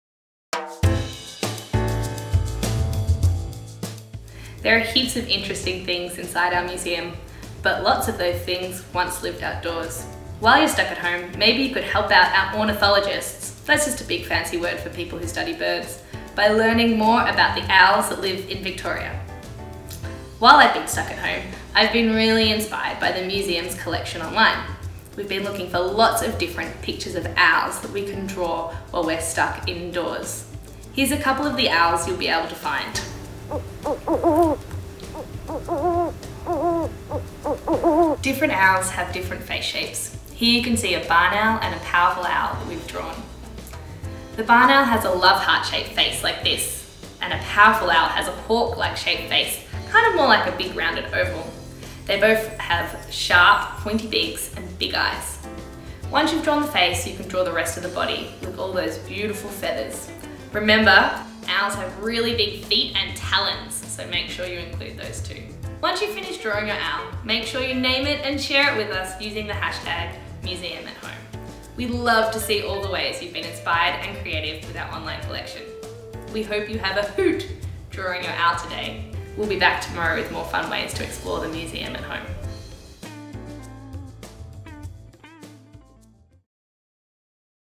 Let's draw an owl home activity
There are heaps of interesting things that we keep inside our museums, but lots of those things once lived outdoors!

While you’re stuck at home, maybe you can help out our Ornithologists (a big fancy word for people who study birds) by learning more about the owls that live in Victoria.

Pick your favourite owl via Collections Online or the Field Guide to Victorian Fauna (app):

Get crafty making your own masterpiece 🎨 Share your creation with us by tagging us or using